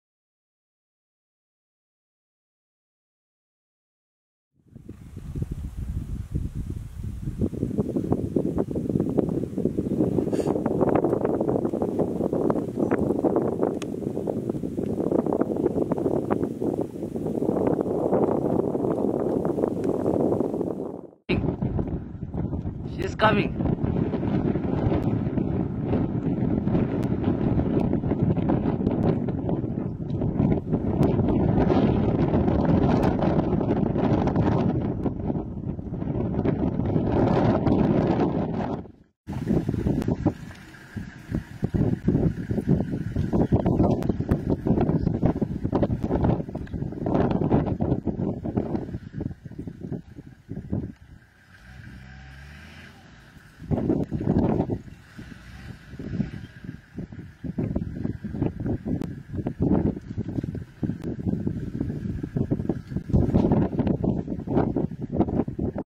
Cheetah hunts a fast gazelle
One of these felines' most prominent characteristics is their light bodyweight, which helps them gaining speed faster. On top of that, they have small heads and long legs for getting the best out of aerodynamics' law. Also, their flexible spine allows for extreme extension during the run.

Filmed in Masai Mara via ol kinyei conservancy